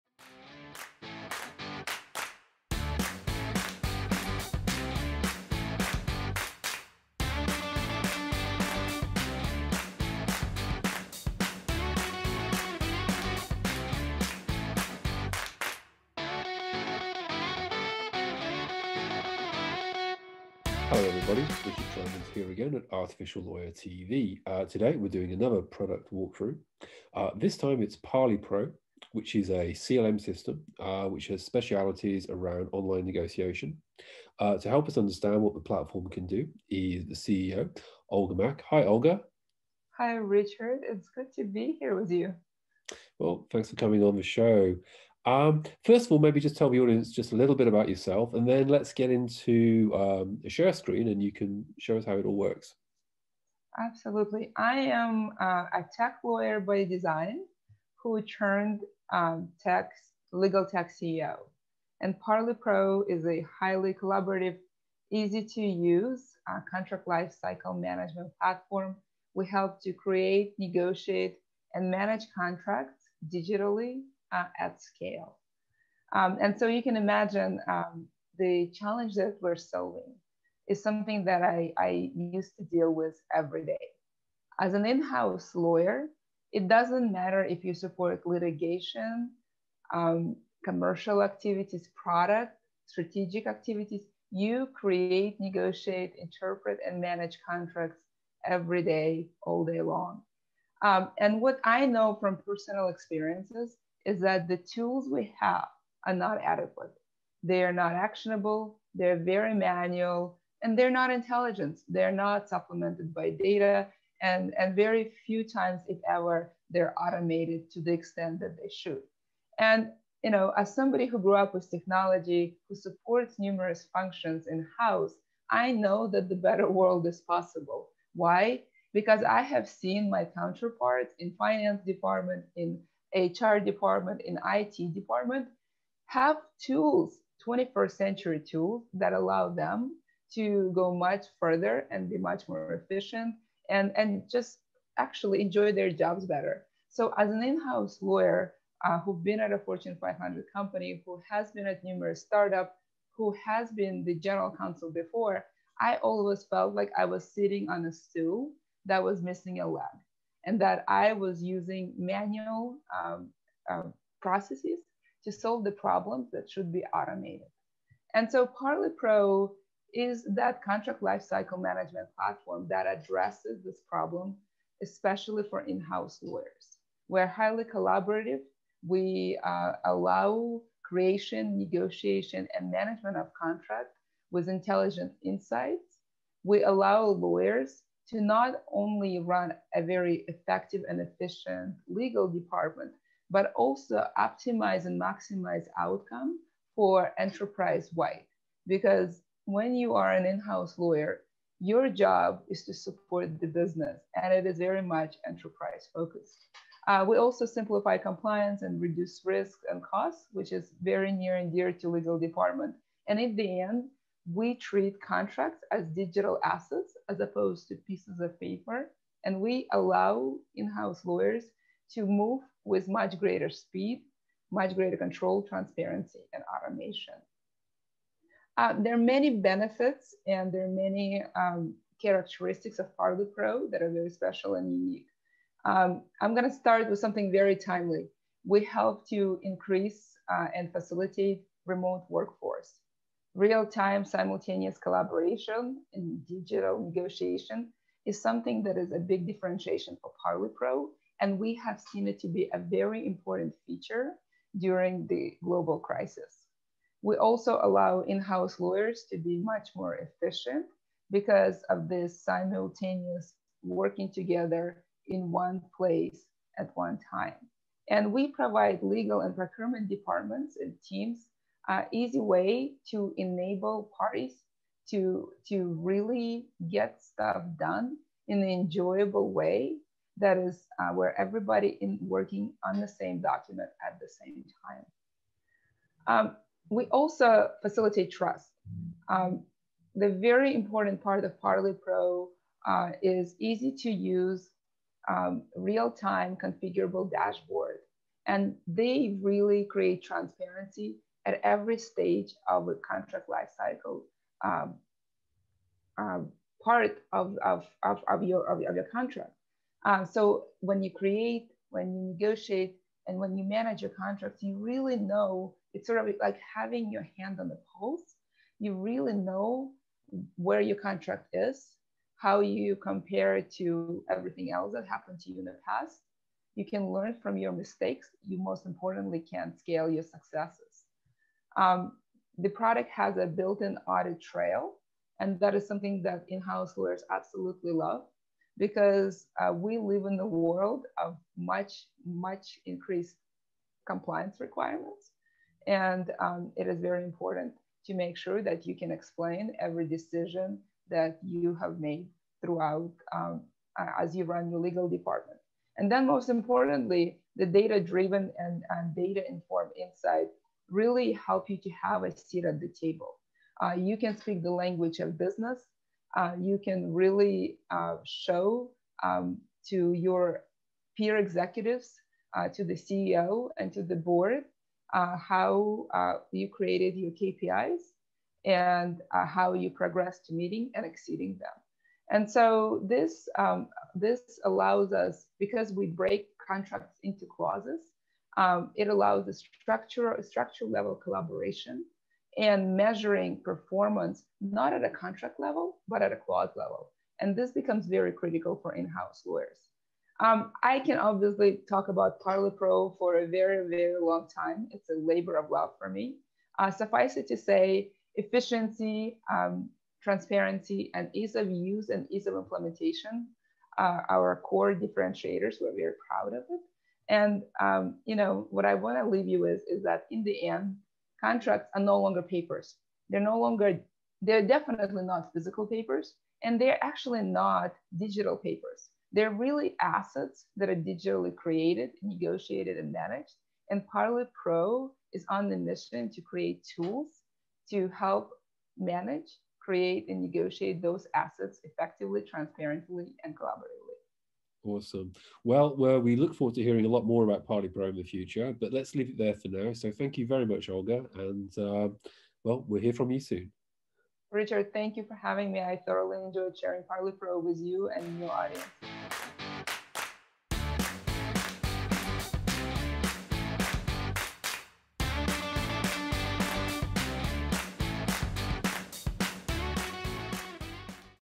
Product Walk Through: Overview - Parley Pro
Parley Pro is a collaborative contract management platform that has a wide range of capabilities, ranging from review and negotiation, to use of templates, to contract workflow management, to use of NLP for key data extraction from contracts, and more.
In this AL TV Product Walk Through, Parley Pro CEO, Olga Mack, provides an overview of what the company does and introduces some of its many capabilities.